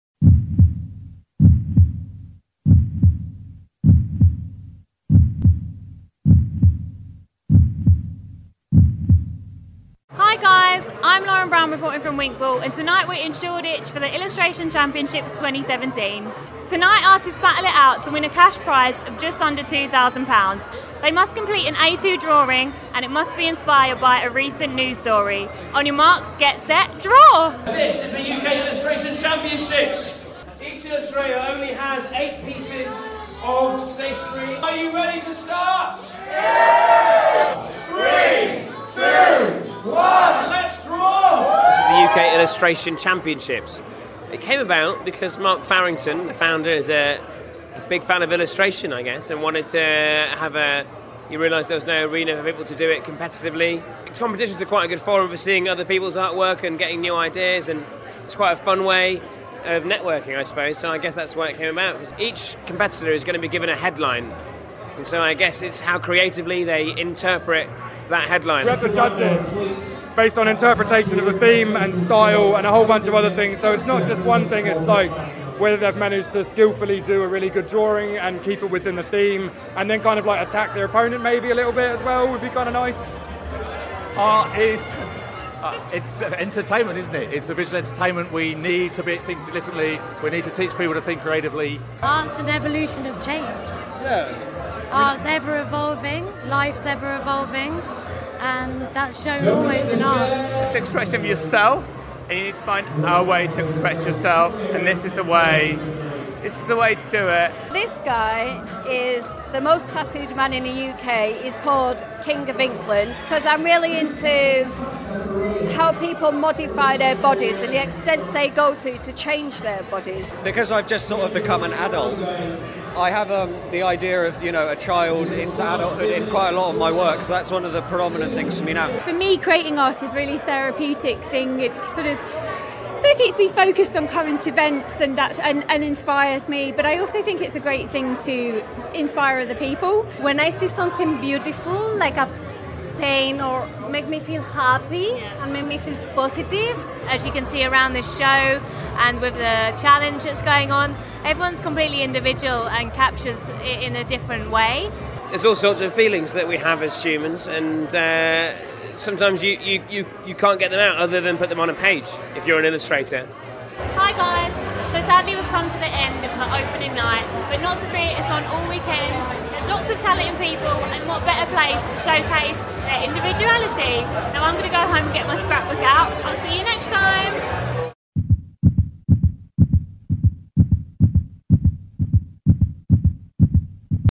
UK Illustration Championship 2017 by WinkBall
UK Illustration Championship brings together a group of the brightest visionaries in contemporary illustration and street art. It was a riveting live drawing battle experience and we were there to capture the action. for more interviews